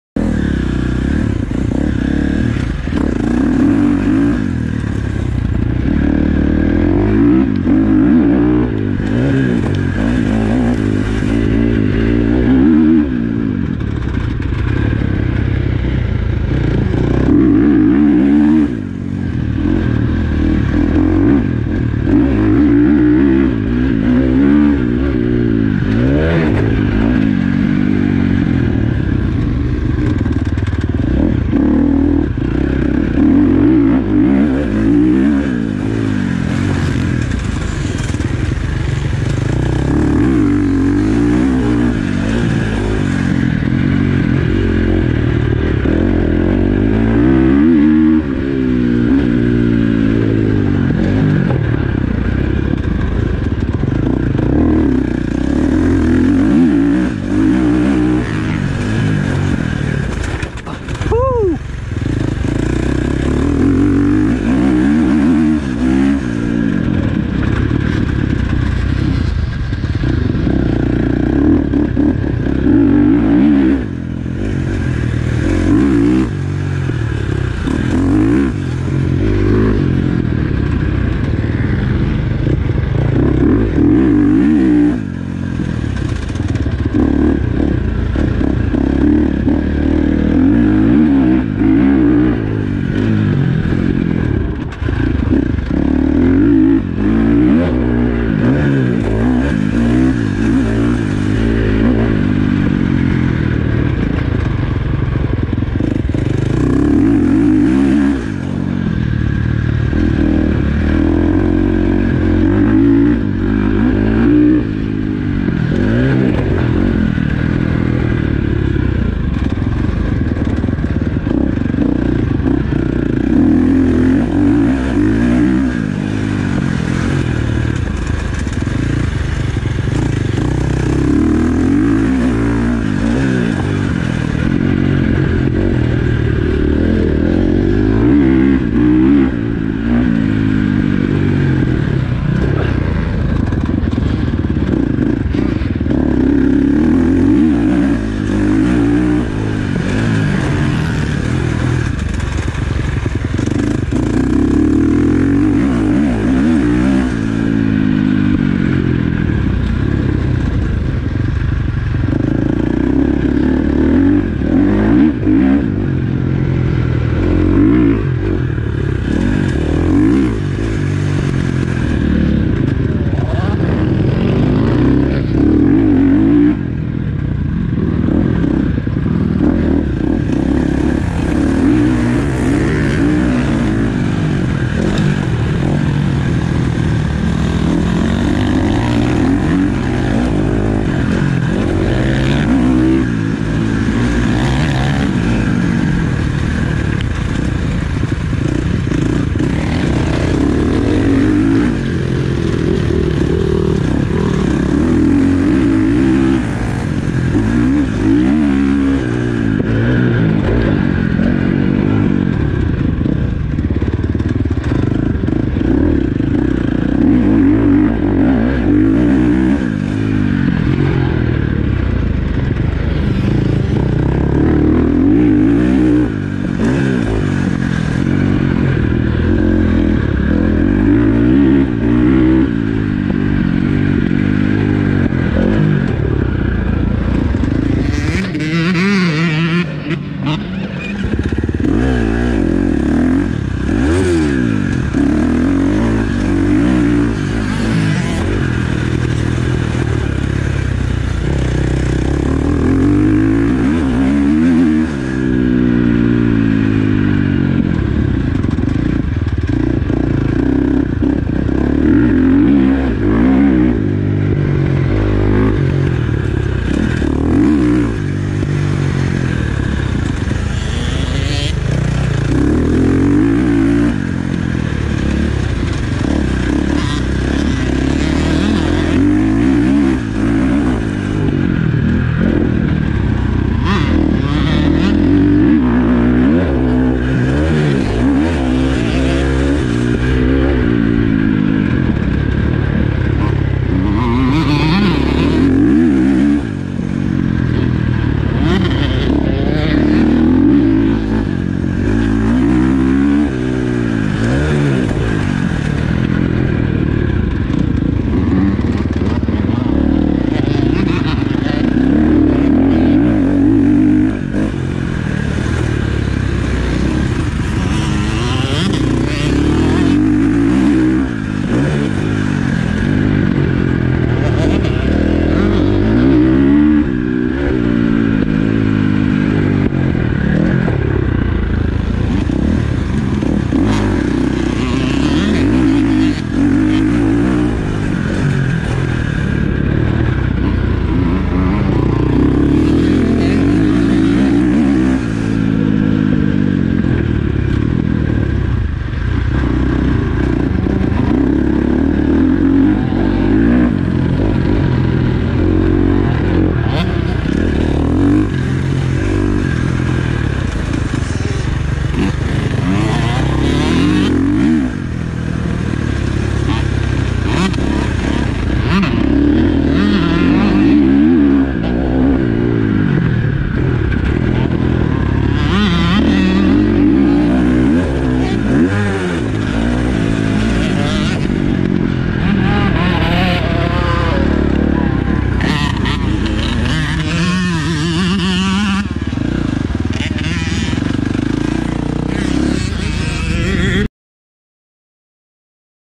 fast traxx (wet) husqvarna 350f
back on me bike again after a mental health breakdown, not gonna lie I was sweating buckets but feels good to get out again 😎 track is very technical but was fun